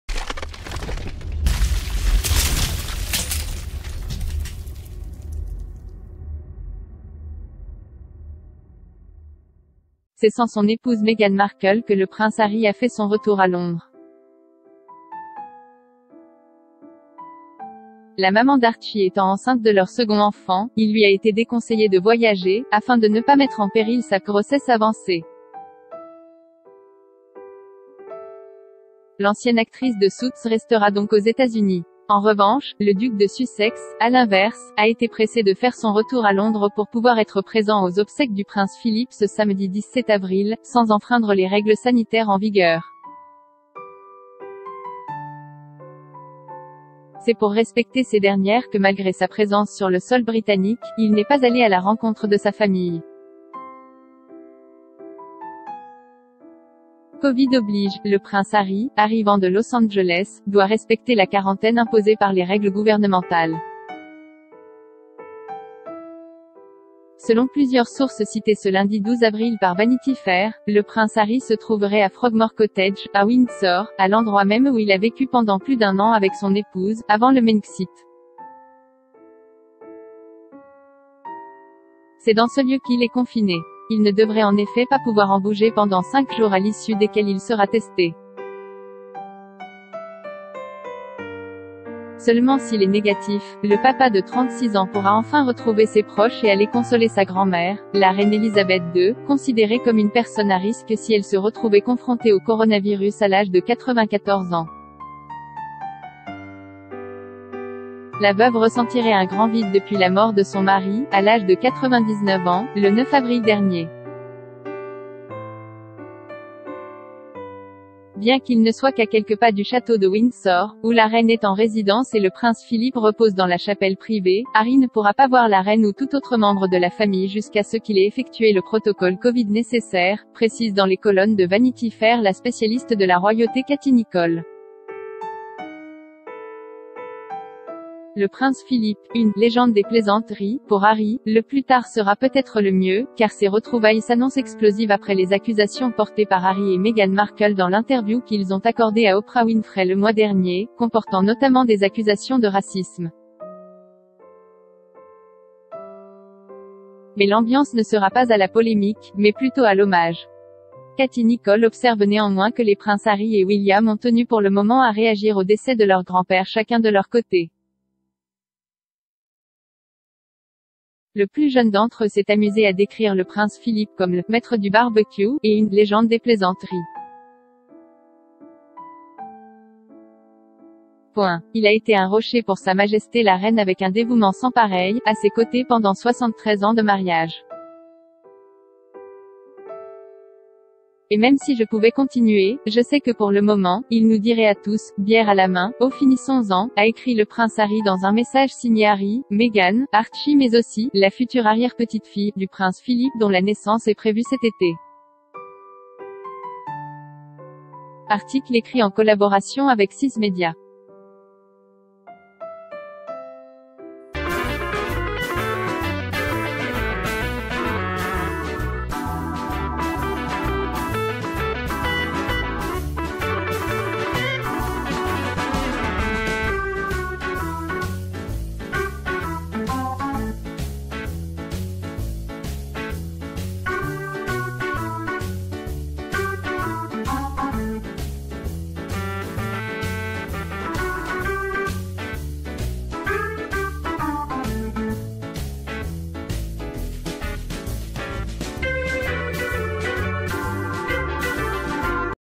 ✅ Pourquoi le prince Harry n’a pas vu sa famille depuis son arrivée à Londres ?
✍ C’est sans son épouse Meghan Markle que le prince Harry a fait son retour à Londres.  La maman d’Archie étant enceinte de leur second enfant, il lui a été déconseillé de voyager, afin de ne pas mettre en péril sa « grossesse avancée ».  L’ancienne actrice de Suits restera donc aux États-Unis. En revanche, le duc de Sussex, à l’inverse, a été pressé de faire son retour à Londres pour pouvoir être présent aux obsèques du prince Philip ce samedi 17 avril, sans enfreindre les règles sanitaires en vigueur.  C’est pour respecter ces dernières que malgré sa présence sur le sol britannique, il n’est pas allé à la rencontre de sa famille.  Covid oblige, le prince Harry, arrivant de Los Angeles, doit respecter la quarantaine imposée par les règles gouvernementales.  .Selon plusieurs sources citées ce lundi 12 avril par Vanity Fair, le prince Harry se trouverait à Frogmore Cottage, à Windsor, à l’endroit même où il a vécu pendant plus d’un an avec son épouse, avant le Megxit.  C’est dans ce lieu  ...